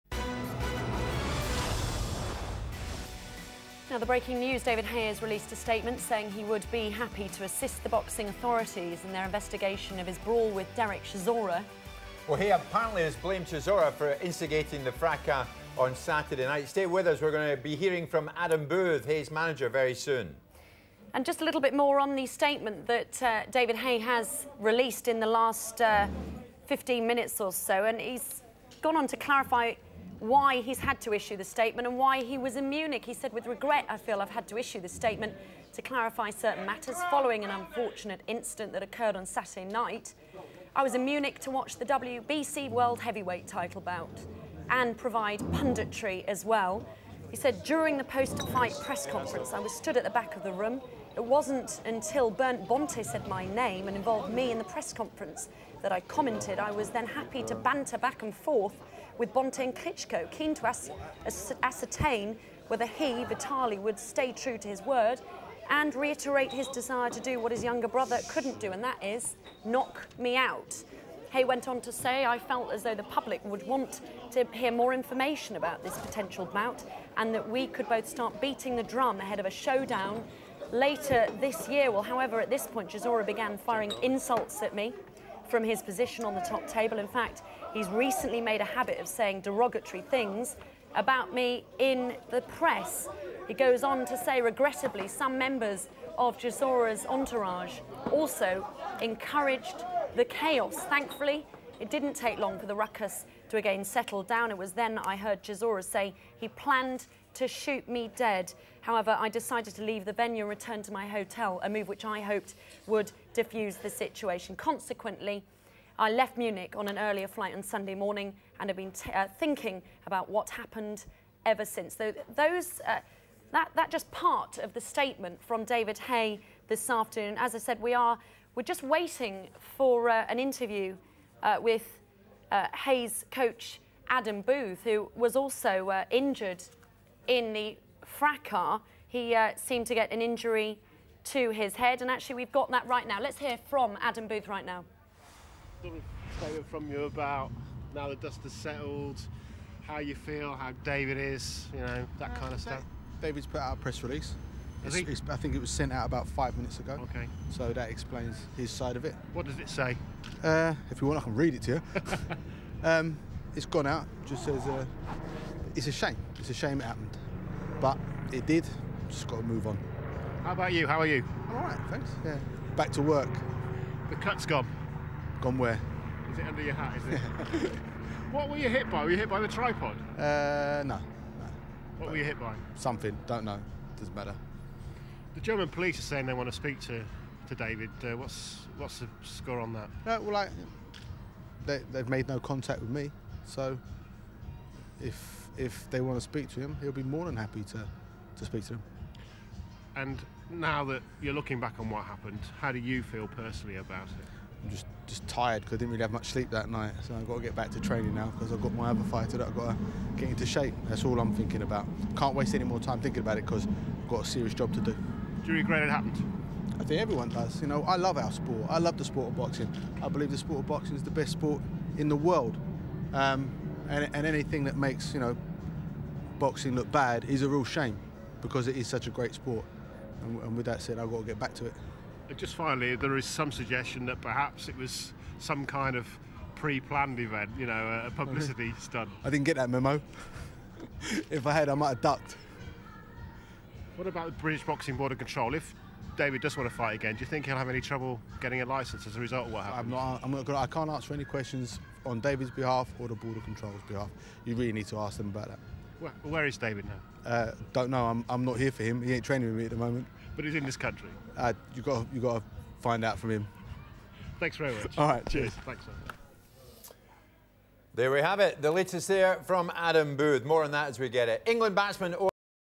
David Haye statement #3 / Adam Booth: Chisora clashes with David Haye
This piece first broadcast on 20 Feb 2012. Televised on UK's satillite television Sky Sports News.